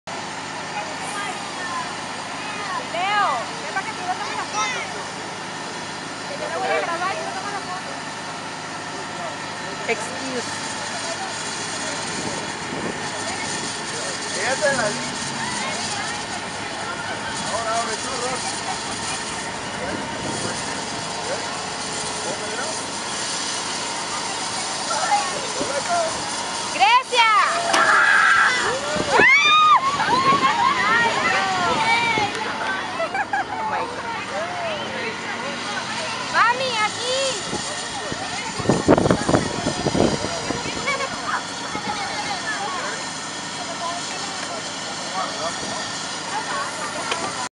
darah.durell da fireboy/girl 08.08.15
tour around da firetruck, learning how it works & finally water...water...water...